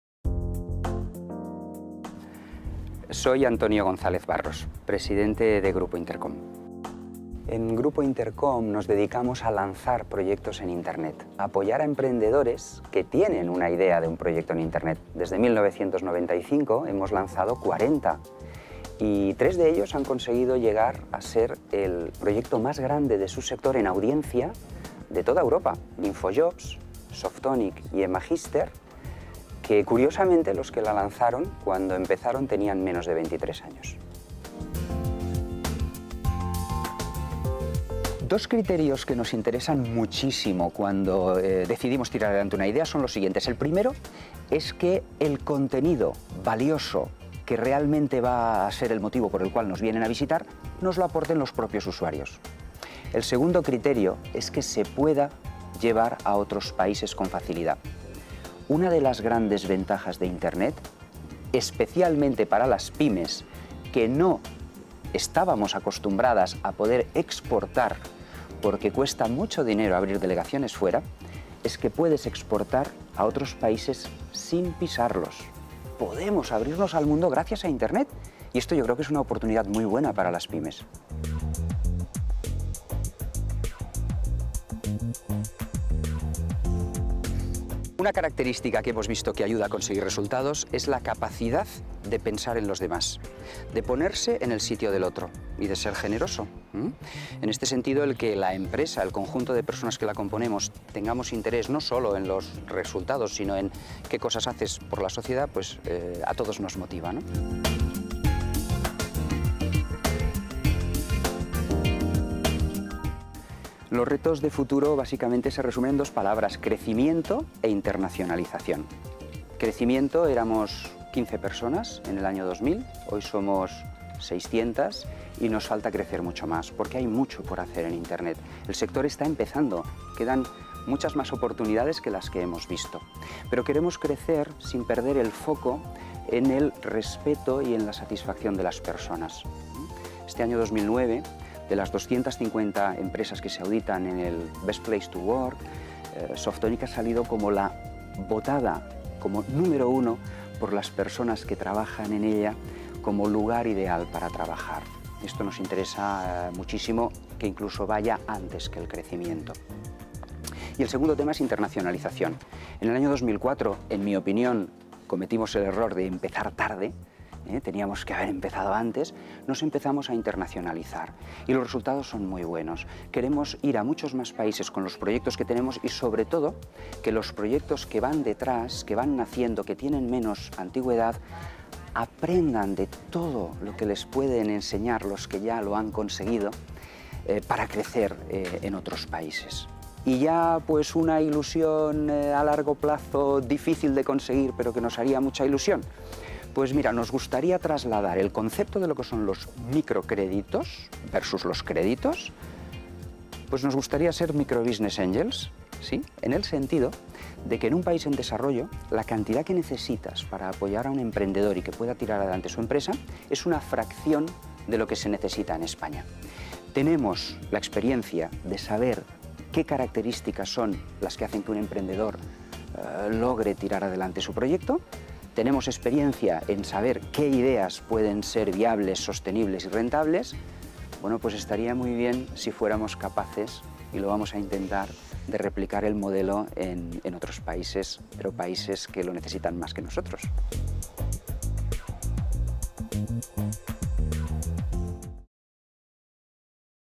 Grupo Intercom: vídeo institucional
Intercom nos da a conocer las ideas de negocio más innovadoras en Internet.

Su Presidente, Antonio González Barros, nos explica su modelo de negocio y cuál es el secreto del enorme éxito de sus más de 30 portales.
Se dedica a lanzar proyectos en Internet, a apoyar emprendedores que tienen una idea de un proyecto en Internet. Tres de ellos han conseguido llegar a ser el proyecto más grande de su sector, en audiencia, de toda Europa: Infojobs, Softonic y Emagister.

Para decidir tirar adelante una idea, tienen en cuenta dos criterios importantes: que el contenido lo aporten los propios usuarios y que se pueda llevar a otros países con facilidad.
Una de las grandes ventajas de Internet, especialmente para las Pymes, es que pueden exportar a otros países sin pisarlos.